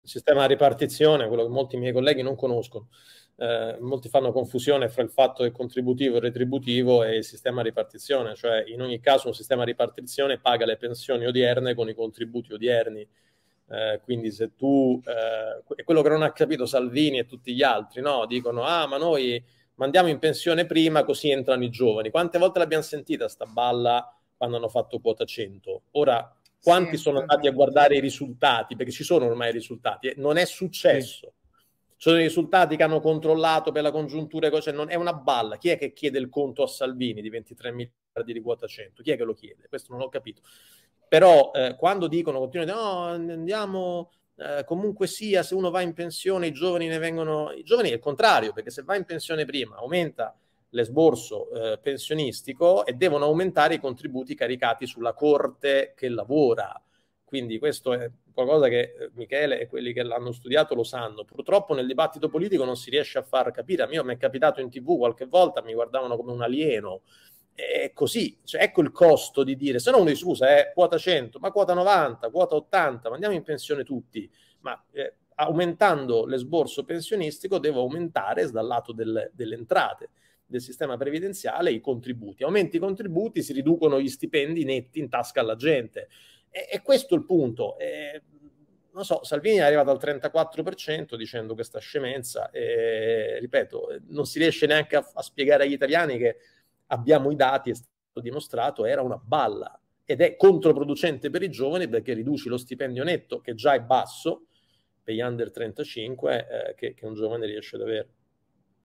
La falsa retorica dei prepensionamenti (Luigi Marattin)
"Mandiamo in pensione prima, così possiamo assumere più giovani" è uno slogan elettorale falso ma duro a morire. Il sistema pensionistico italiano è a ripartizione. Ciò significa che le pensioni odierne vengono pagate dai contributi dei lavoratori odierni. In aggiunta a ciò, l'Italia è da decenni in declino demografico. Ergo se i pensionamenti aumentano, il sistema pensionistico aumenta di peso e deve essere aumentato il carico di contributi preso da chi lavora, quindi diminuendo i salari netti. Ecco perché quota 100 non ha funzionato e ci sono i dati a dimostrarlo. Mandare in pensione prima non beneficia i giovani, li danneggia. La "staffetta generazionale" è solo retorica.